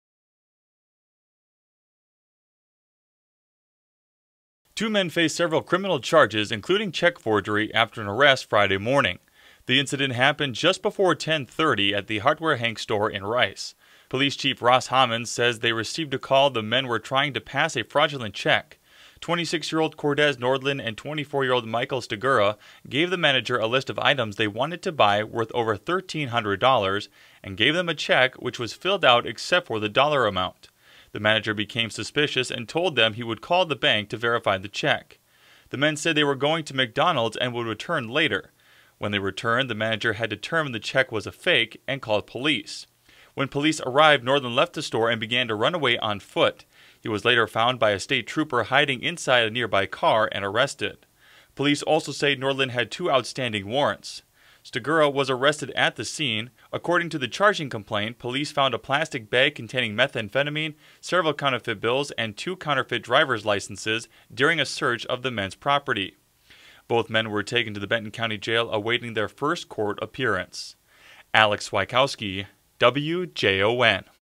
Two Men Face Criminal Charges in Check Forgery Bust [VIDEO]
Two men face several criminal charges, including check forgery, after an arrest Friday morning.